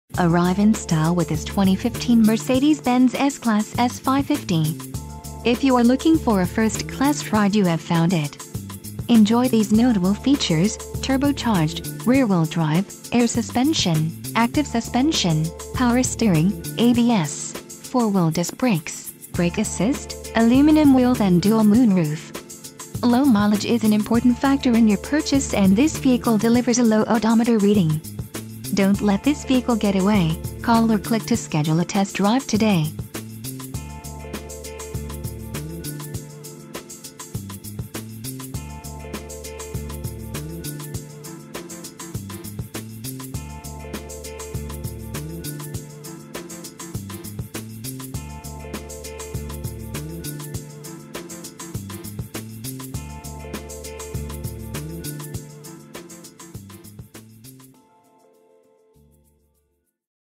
2015 Mercedes-Benz S-Class S550 - Mercedes-Benz Daytona B...
2015 Mercedes-Benz S-Class S550 Condition: New Transmission: 7-Speed Automatic w/OD Exterior Color: Gray Interior Color: Tan Doors: 4 Stock: FA117152 Cylinders: 8